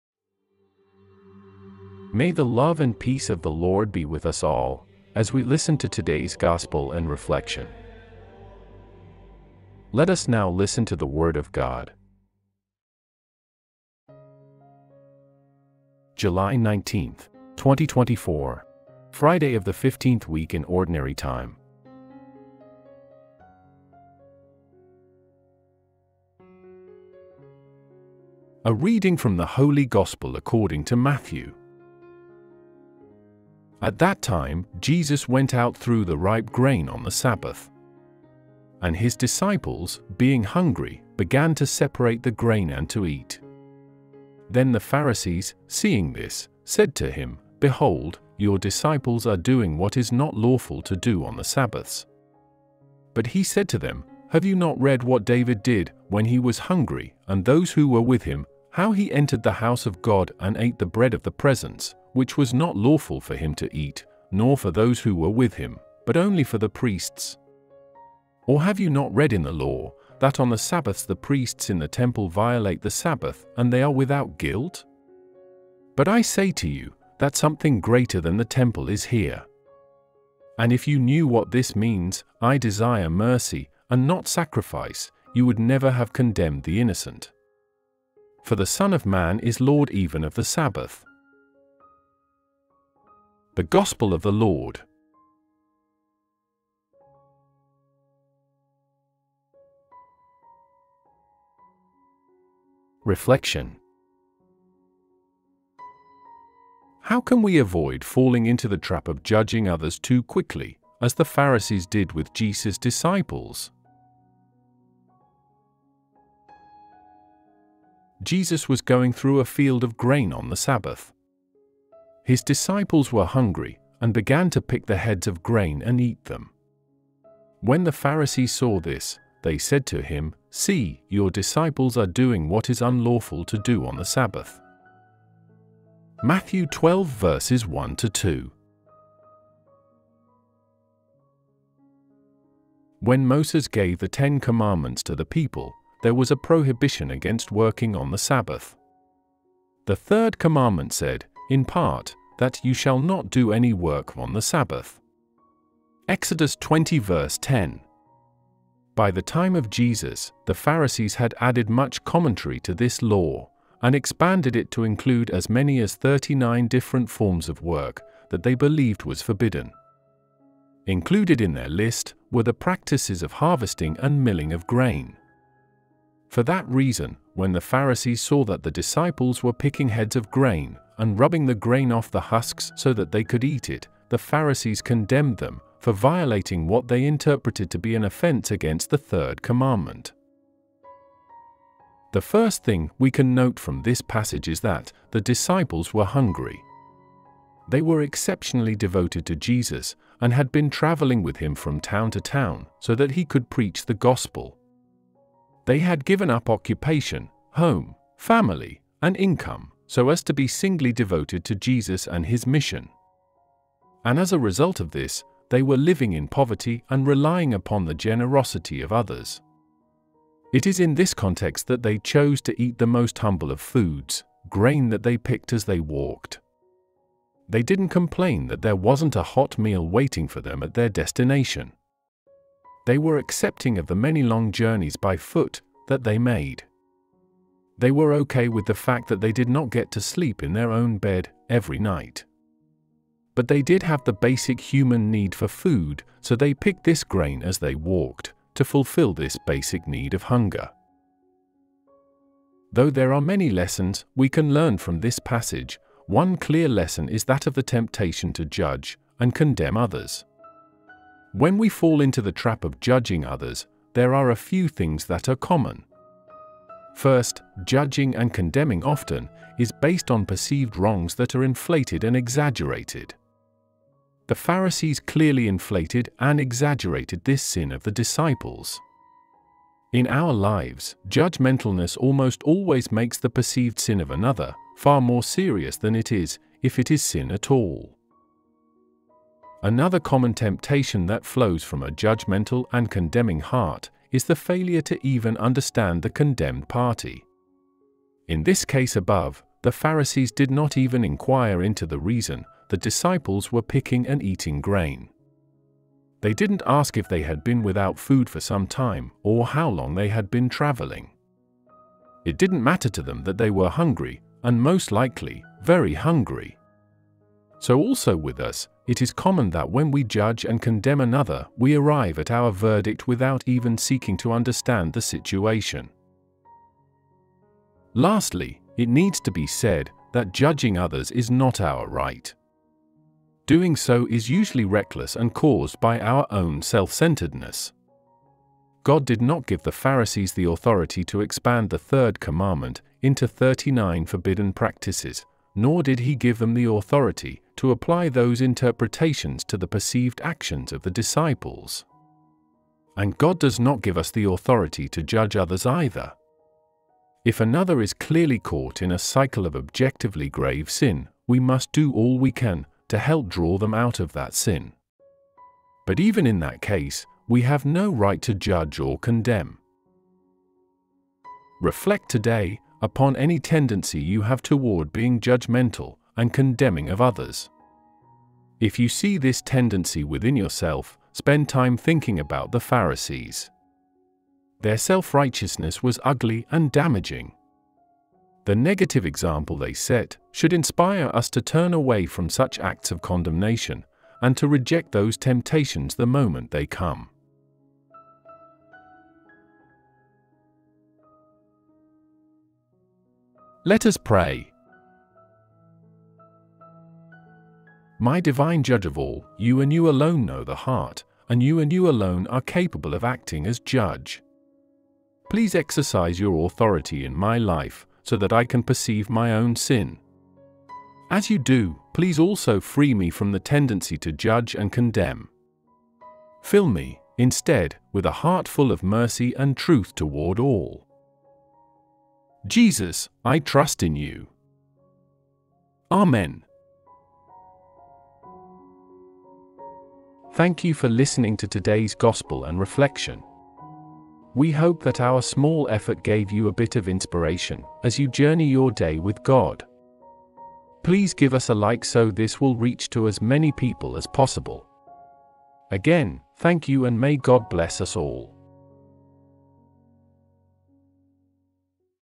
Today's Catholic Mass Gospel and Reflection for July 19, 2024 - Matthew 12:1-8
Mercy on the Sabbath

Friday of the Fifteenth Week in Ordinary Time
Lectionary: 393

Alleluia: Jn 10:27
R. Alleluia, alleluia.
My sheep hear my voice, says the Lord;

At that time, Jesus went out through the ripe grain on the Sabbath. And his disciples, being hungry, began to separate the grain and to eat. Then the Pharisees, seeing this, said to him, “Behold, your disciples are doing what is not lawful to do on the Sabbaths.” But he said to them: “Have you not read what David did, when he was hungry, and those who were with him: how he entered the house of God and ate the bread of the Presence, which was not lawful for him to eat, nor for those who were with him, but only for the priests? Or have you not read in the law, that on the Sabbaths the priests in the temple violate the Sabbath, and they are without guilt? But I say to you, that something greater than the temple is here. And if you knew what this means, ‘I desire mercy, and not sacrifice,’ you would never have condemned the innocent. For the Son of Man is Lord even of the Sabbath.”

Friday, July 19, 2024 - A moment with GOD through HIS WORD.

Reflection: How can we avoid falling into the trap of judging others too quickly, as the Pharisees did with Jesus' disciples?

Prayer: My divine Judge of All, You and You alone know the heart, and You and You alone are capable of acting as Judge. Please exercise Your authority in my life so that I can perceive my own sin. As You do, please also free me from the tendency to judge and condemn. Fill me, instead, with a heart full of mercy and truth toward all. Jesus, I trust in You.